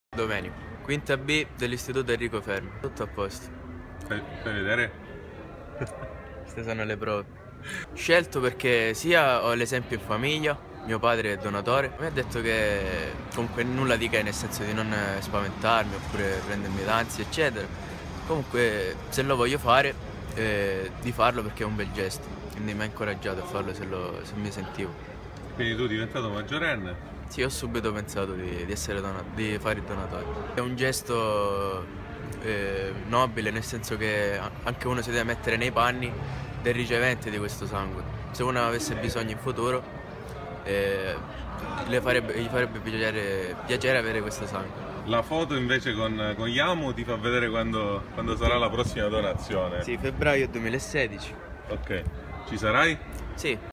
Domenico sulle orme del padre donatore Avis
L'esempio del padre donatore nell'associazione volontari del sangue è stato determinante. Domenico, studente al quinto anno dell'Istituto professionale "Fermi-Boccioni" di Reggio Calabria, ha scelto di compiere questo gesto di solidarietà appena diventato maggiorenne. E di raccontare questa prima volta su IAMU.